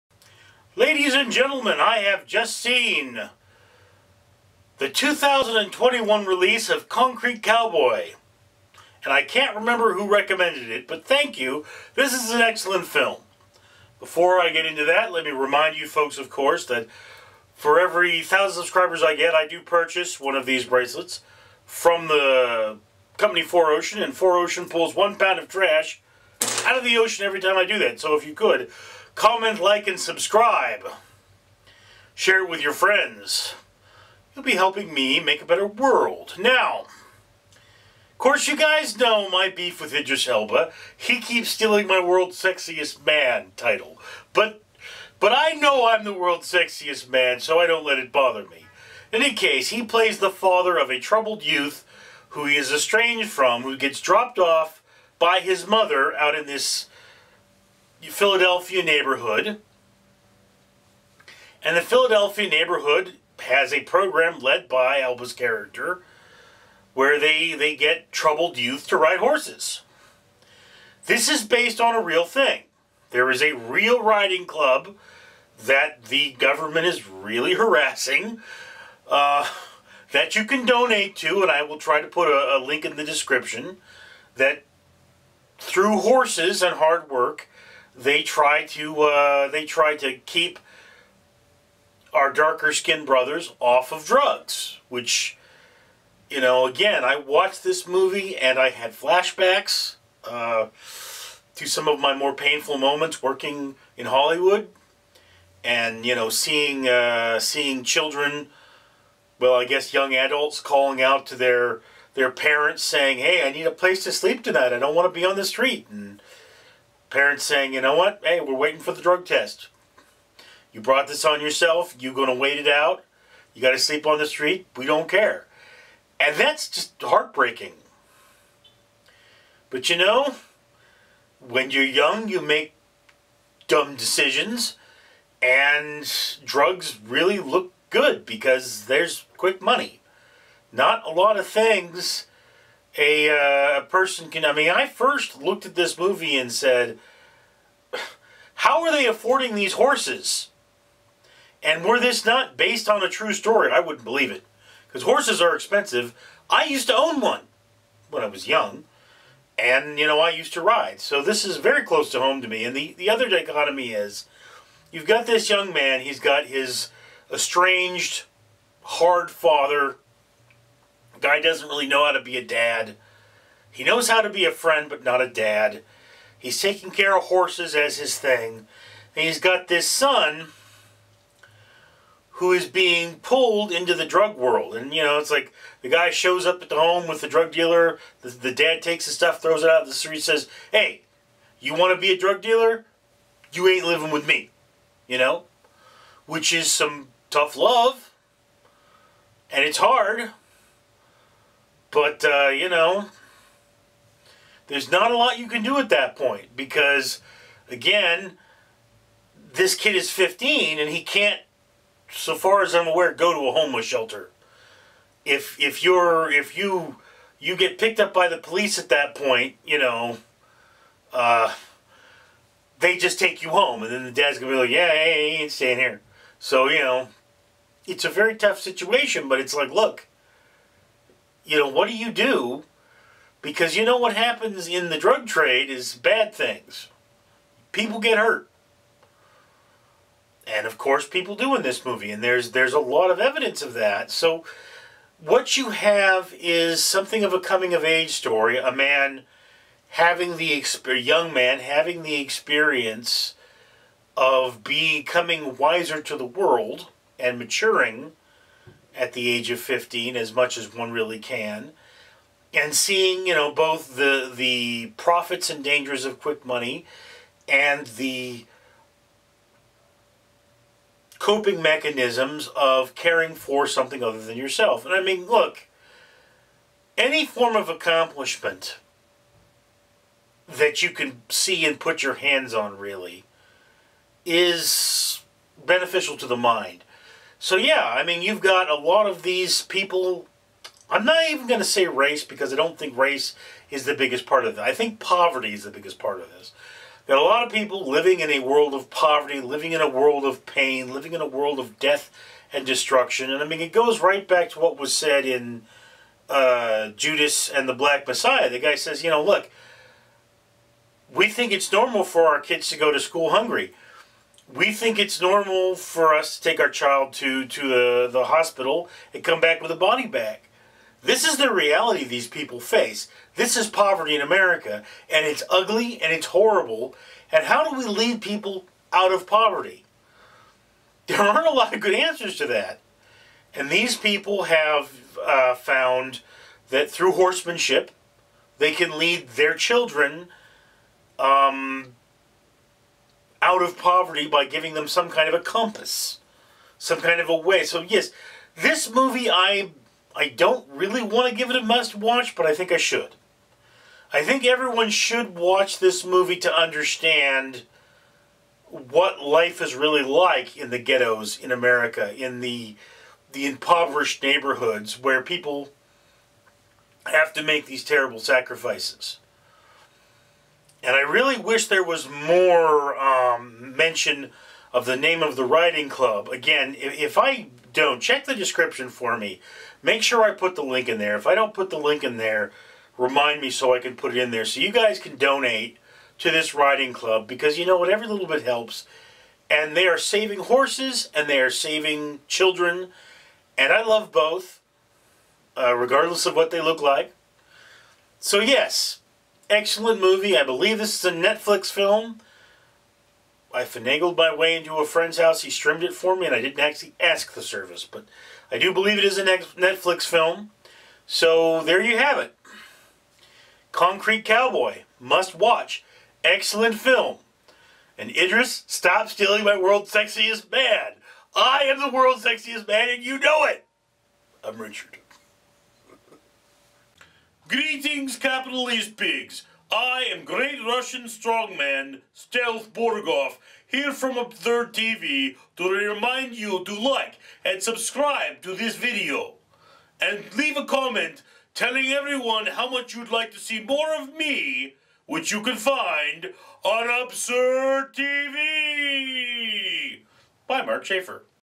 Concrete Cowboy (2020) Movie Review
A young man finds redemption by looking after a hours.